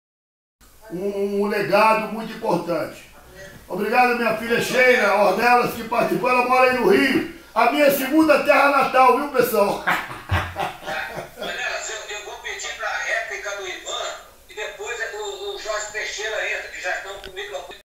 Live Jubileu de Ouro (50 anos) Turma 153 de 70 EEAR - FAB Canarinhos 70 Parte VIII Pauliran Ornelas
Live do Jubileu de Ouro (50 anos) da Turma 153 de 1970 da EEAR - Escola de Especialistas de Aeronáutica - Força Aérea Brasileira "Canarinhos de 70"